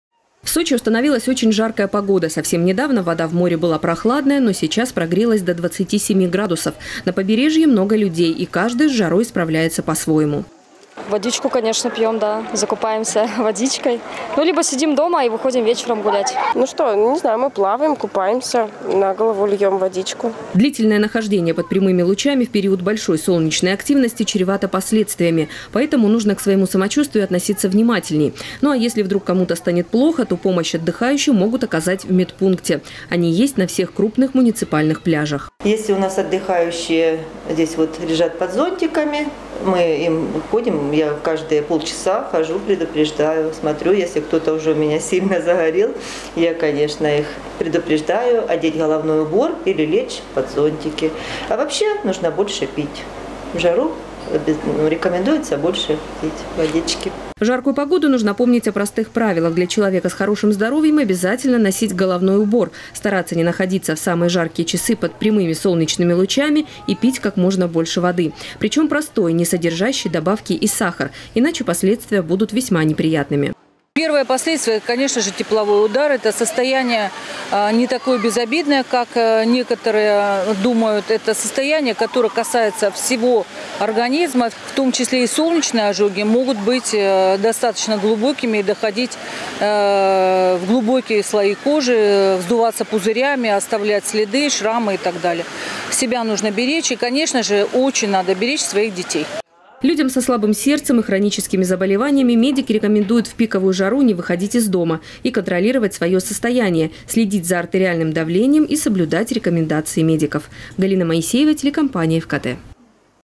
МЧС предупреждает о высокой температуре в Сочи. Новости Эфкате
По прогнозам специалистов, столбик термометра может достигать отметки 37 градусов в тени.

В Сочи установилась жаркая погода. Совсем недавно вода в море была прохладная, но сейчас прогрелась до 27 градусов. Надо помнить, что длительное нахождение под прямыми лучами в период солнечной активности чревато последствиями. На каждом крупном муниципальном пляже Сочи тем, кто перегрелся на солнце, помогут в оборудованных медпунктах.